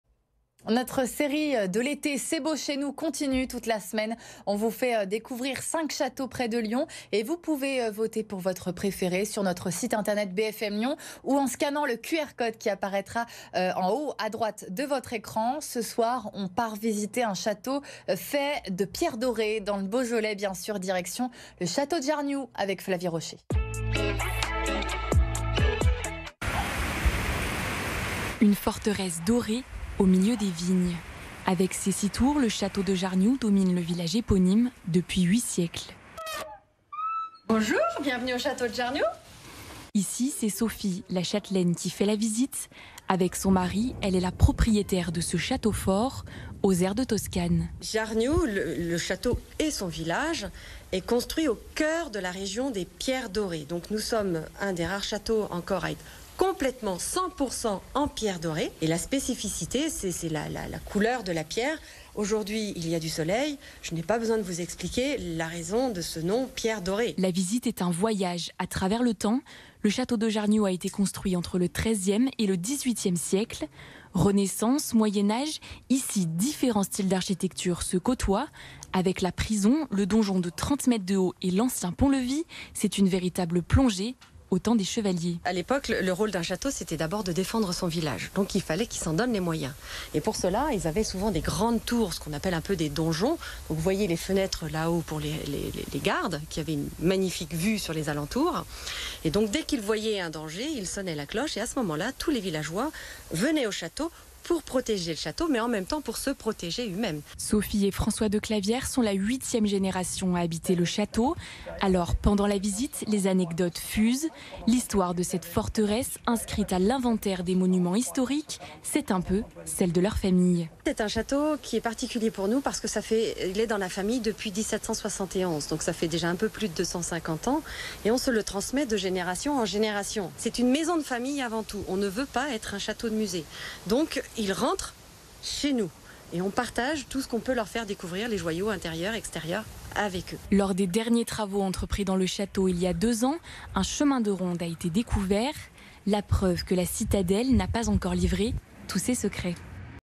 Beaujolais: découverte du château de Jarnioux
Cet été, BFM Lyon vous fait découvrir 5 châteaux près de Lyon. Cette fois, direction le château de Jarnioux dans le Beaujolais. Vous pouvez voter pour votre monument préféré sur le site de BFM Lyon.